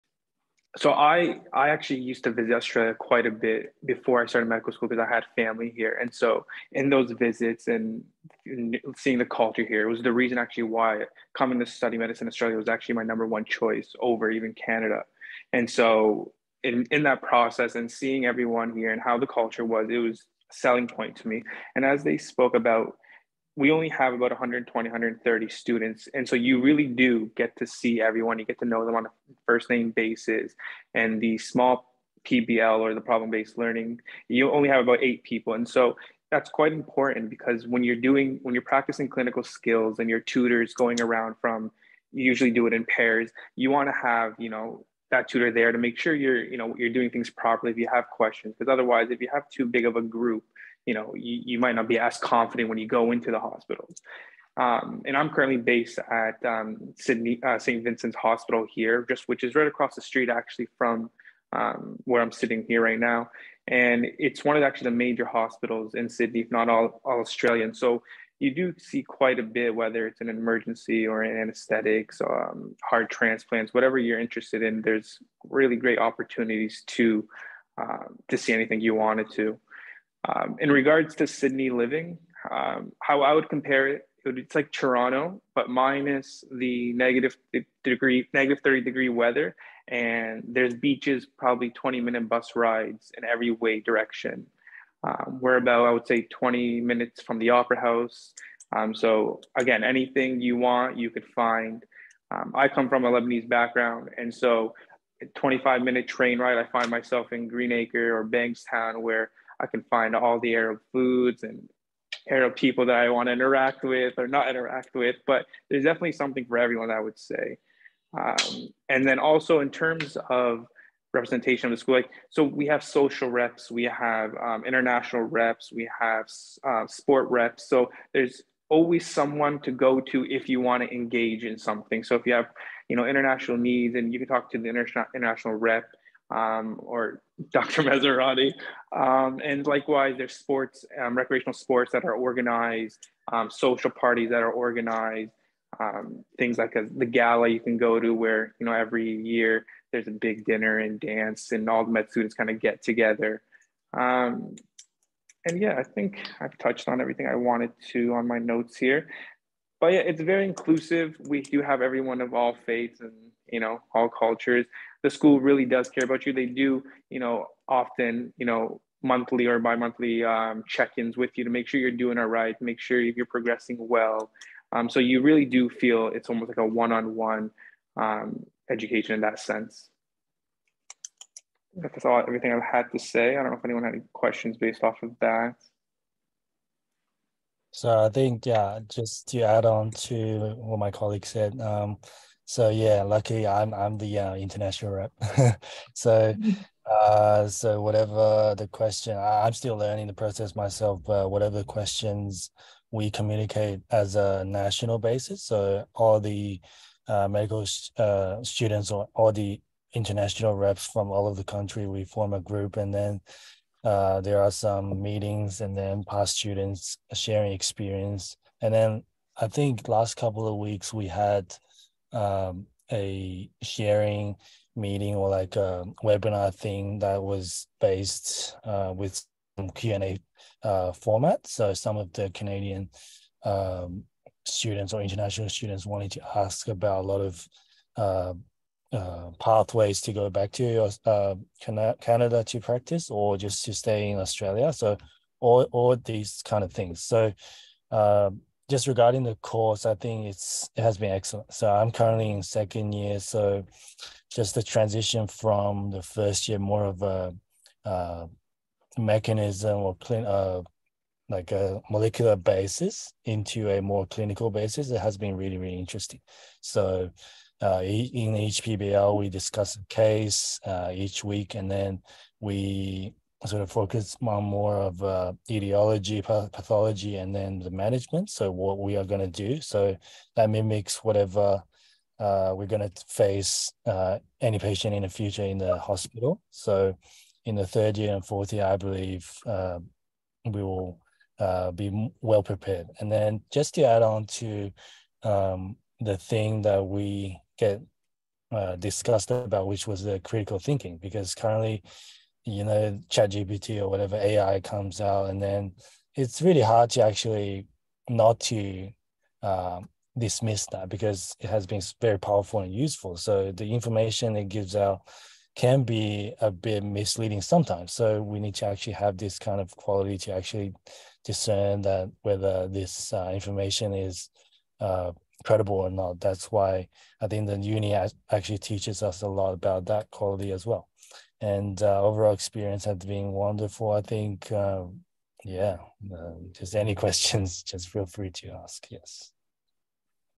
Studying Medicine at The University of Notre Dame Australia: International Student Experience
Meet the University of Notre Dame Australia Doctor of Medicine!

In this clip, hear from two international students about their experiences studying medicine at Notre Dame. They'll give you a glimpse into a bit of everything, from classes and support to what it's like to live in beautiful Australia!

This webinar was recorded April 12, 2023. Would you like to watch the full recording?

To learn more about the University of Notre Dame Australia,